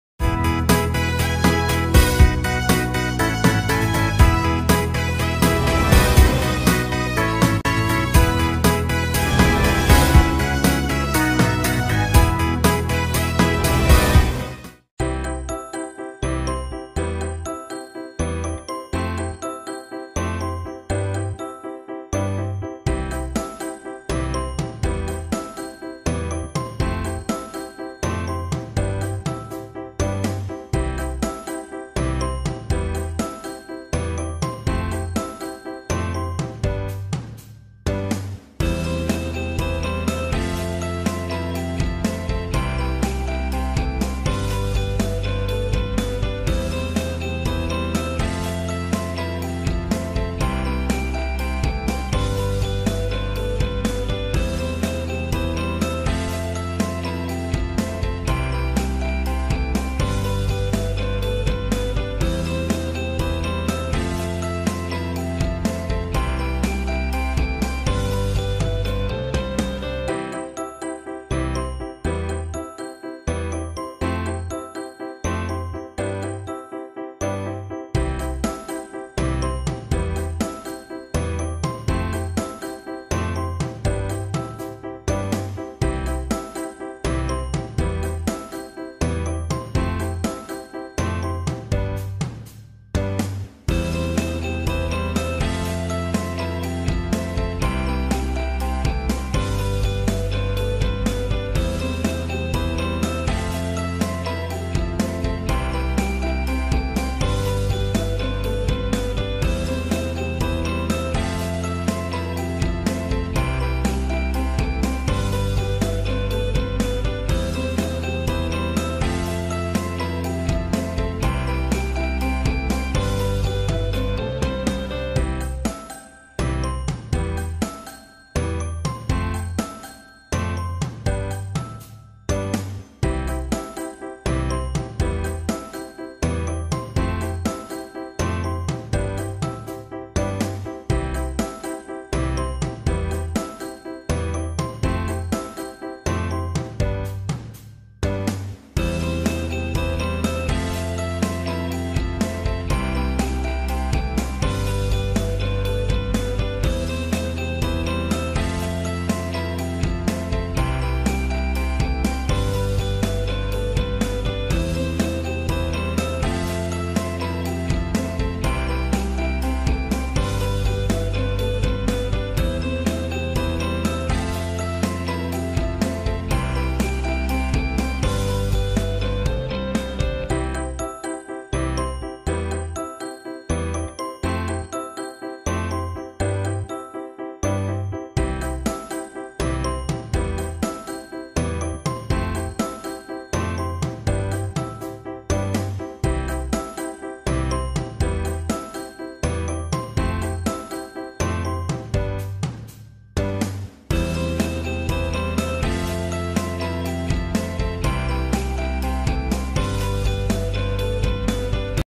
Best Valentines day gift / Heart shape Double Decker Love Box Turtorial / Gift box/ easy crafts
This video shows how to make a valentine gift box